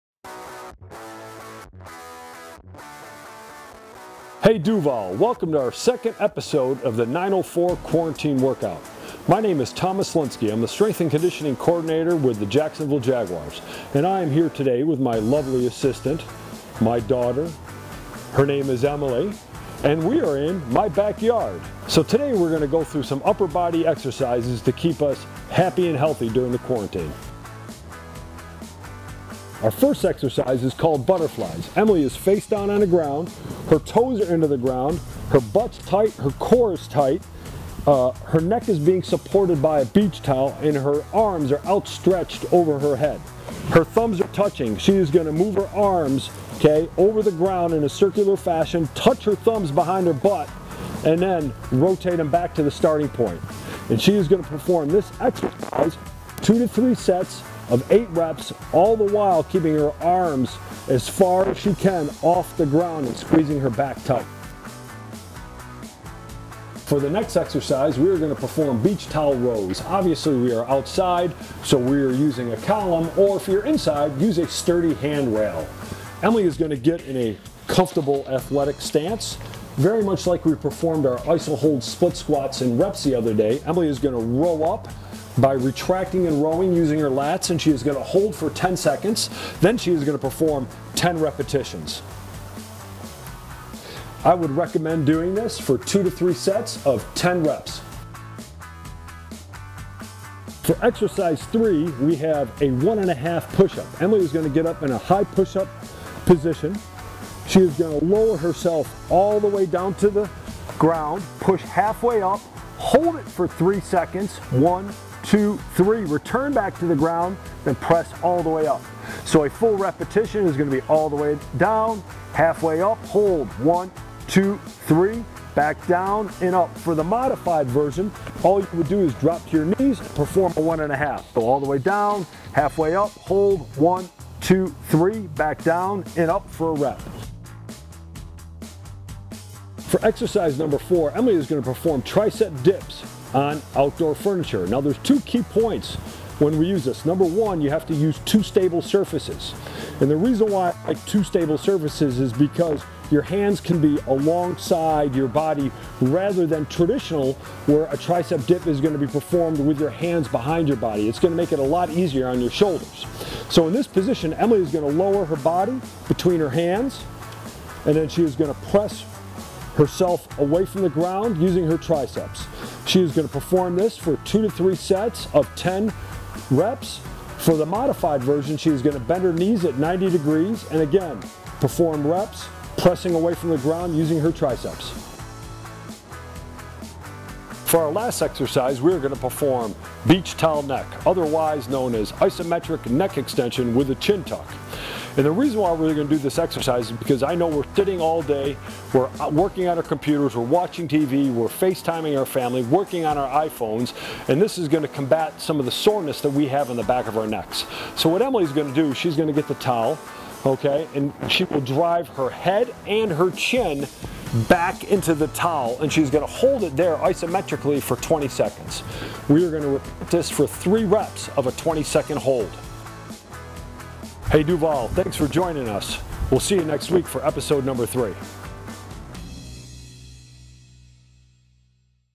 Upper Body Workout | 904 Workouts with Jaguars Strength & Conditioning Coordinator Tom Myslinski
Jaguars Strength & Conditioning Coordinator Tom Myslinski takes you through an upper body workout that anyone can do from home.

Workouts come out on Tuesdays and Thursdays each week.